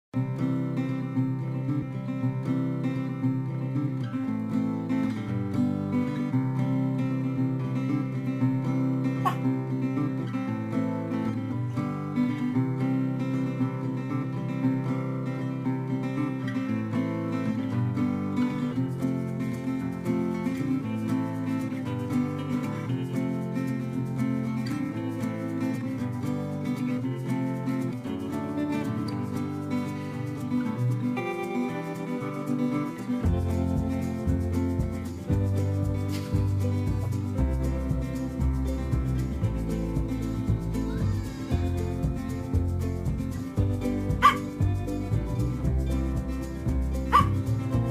마루 냥냥펀치~
렉돌 아기냥이(토니)와 말티즈(마루) 훈육